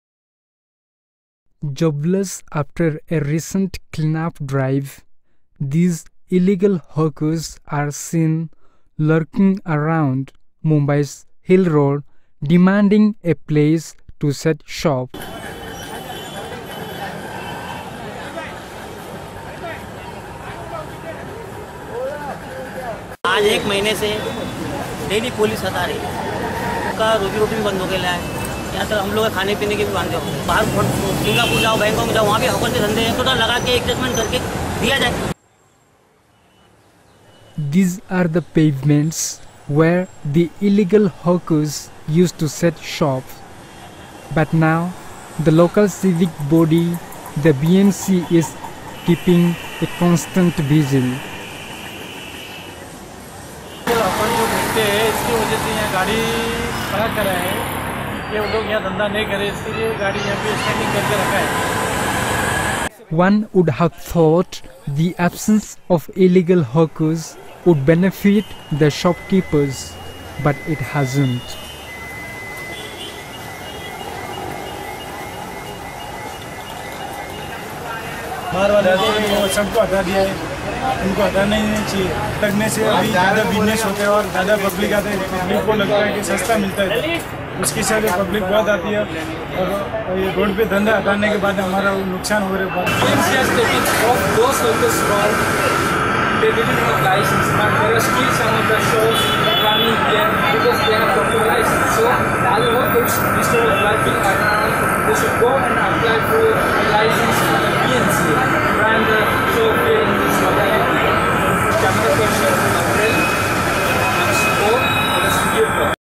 A Hide & Seek Game Between Hawkers and Police-BMC
Reporting by The First Batch of Journalism Students at SPICE